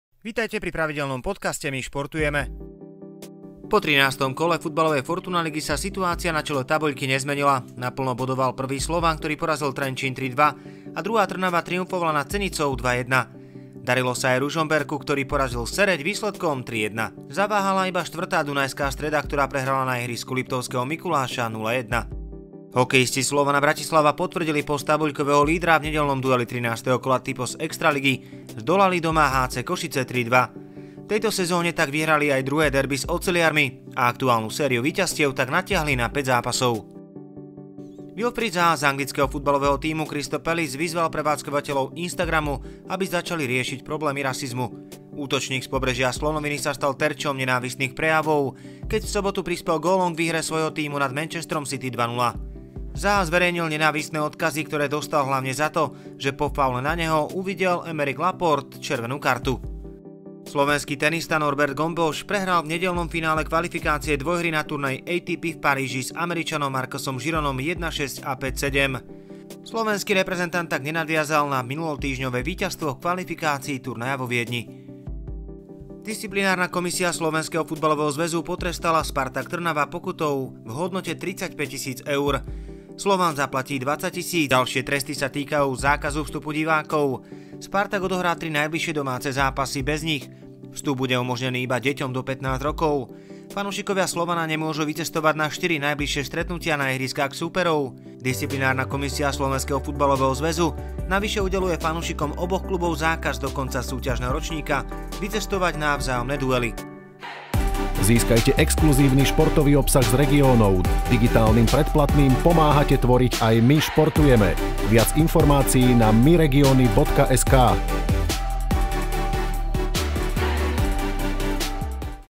Spartak aj Slovan s trestami, Špičkový futbalista poukázal na instagram
Aj dnes sa v MY Športujeme zameriame na čerstvé správy zo sveta slovenského a zahraničného športu. Prezradíme si ako sa darilo futbalovým tímom v najvyššej lige a okrem iného sa zameriame na rozhodnutie disciplinárnej komisie SFZ, ktorá udeľovala tresty po výtržnostiach fanúšikov.